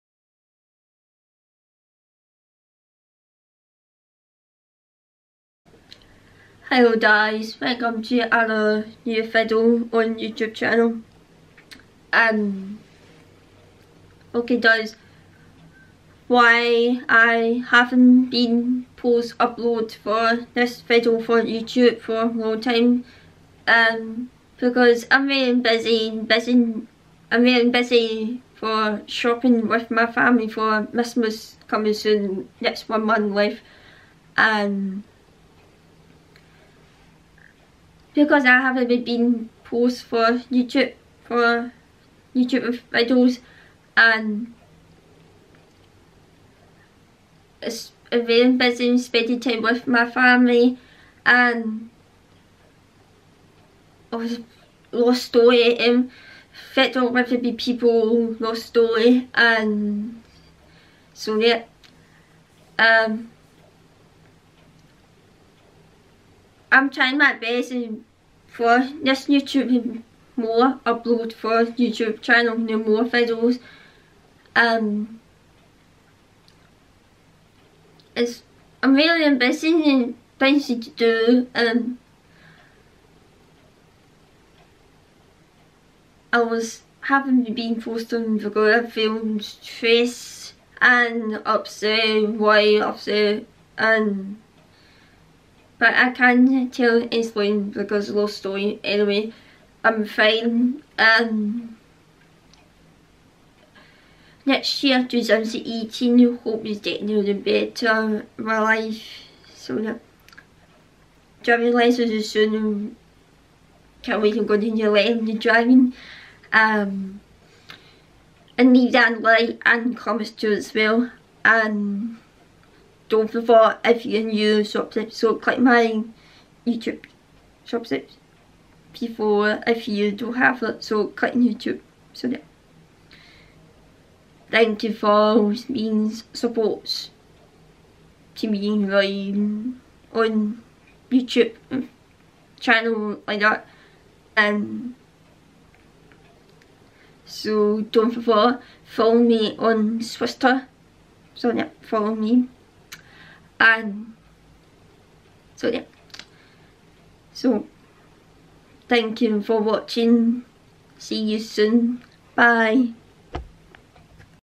Why i haven't been post on youtube video 😞
Have a nice day. any good idea next new video soon.., sorry i feel bad day couple days ago. I'm fine now anyway. See you soon or couple days.